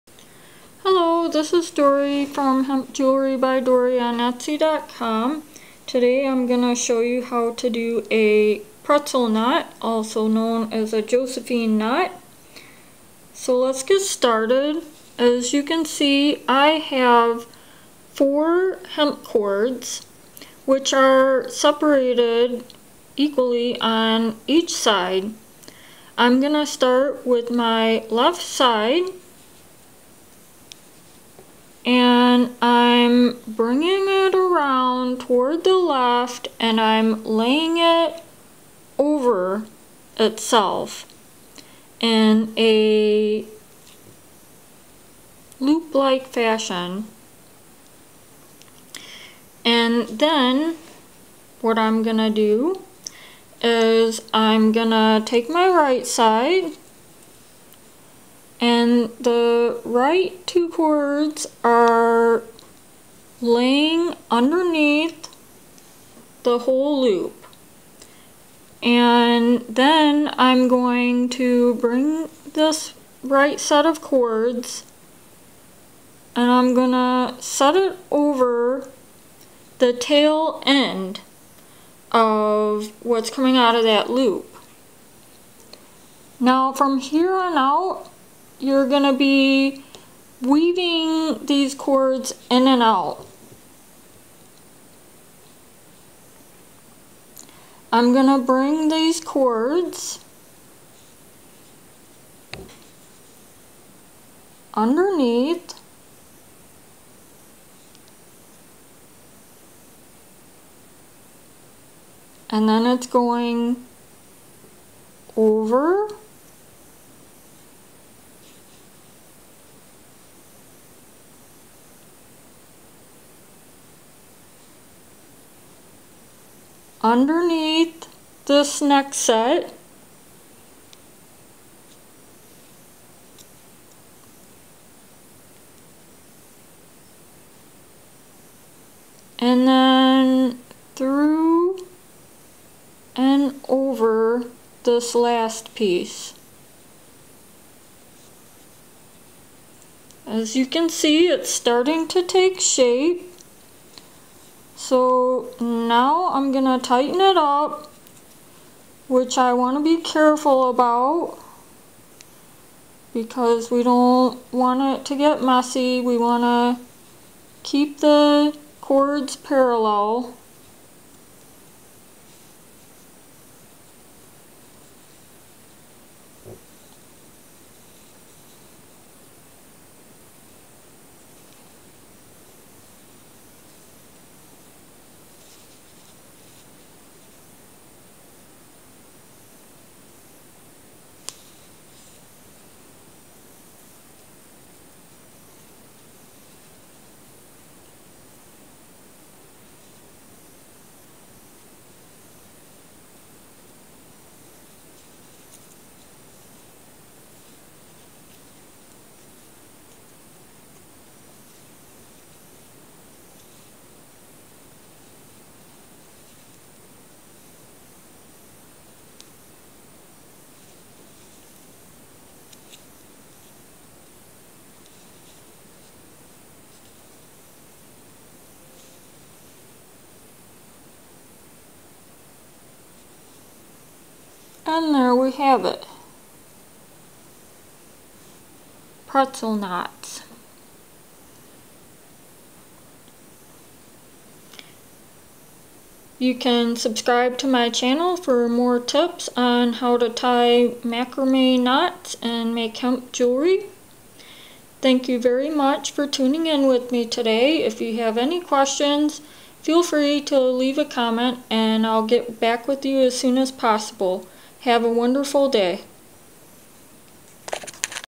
How to Tie a Pretzel AKA Josephine Knot for Hemp Jewelry
Need to stock up on hemp cord? (Paid Link)

A step by step tutorial on how to tie a Pretzel knot for hemp jewelry. You can use this knot to make a hemp necklace, choker, bracelet, or anklet.